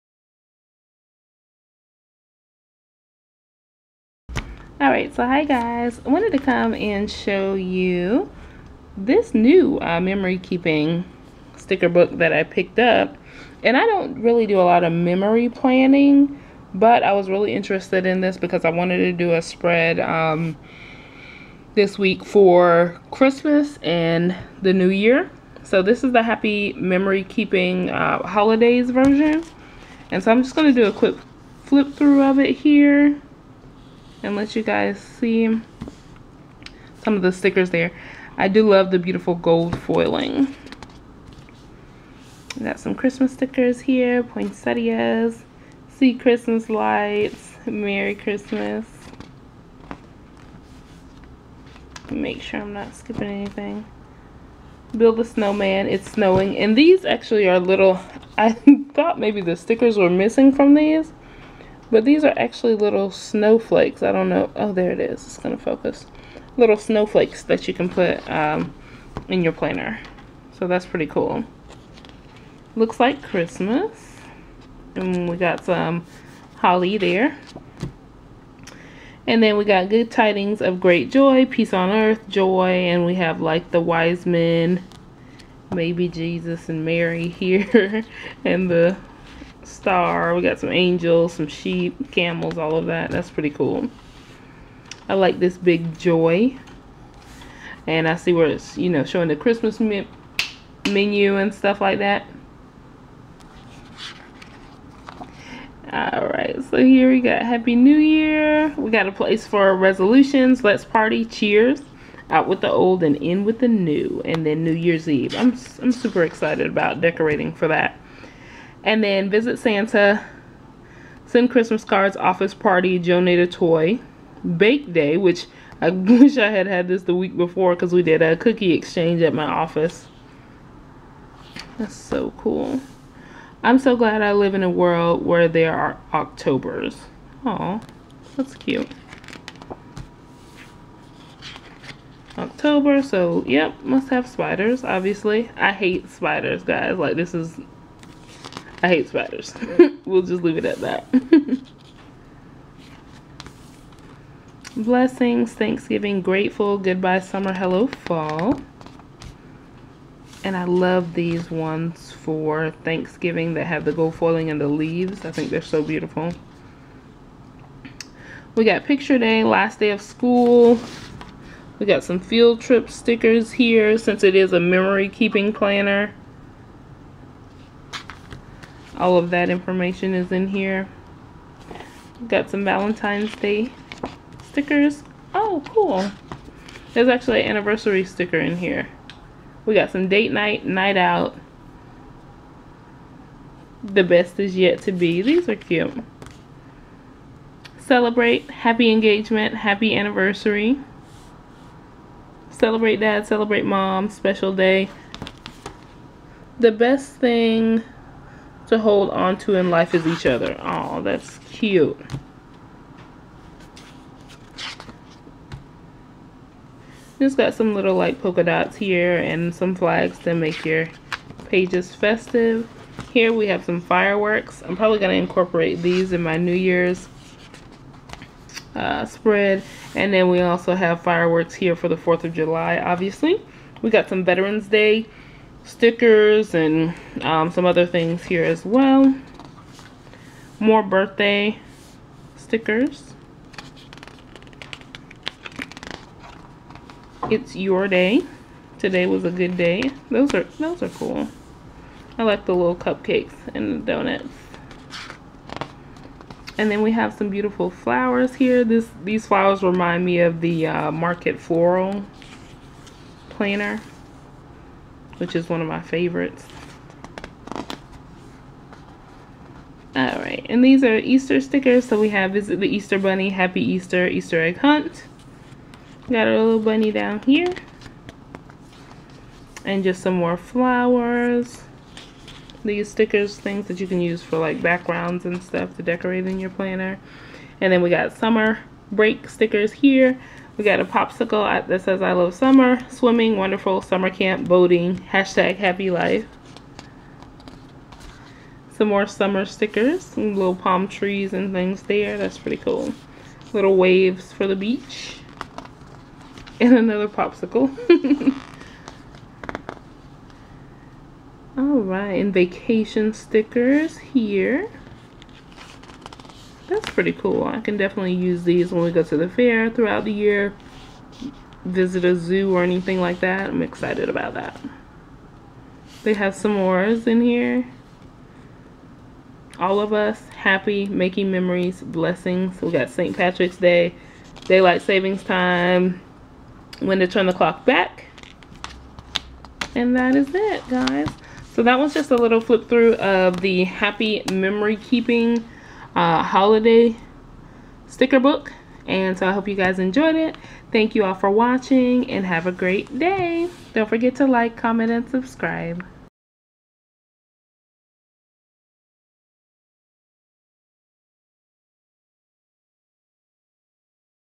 Happy Memory Keeping - Holidays - Stickerbook Flip Through!
Come watch me flip through the Happy Memory Keeping - Holidays - Stickerbook!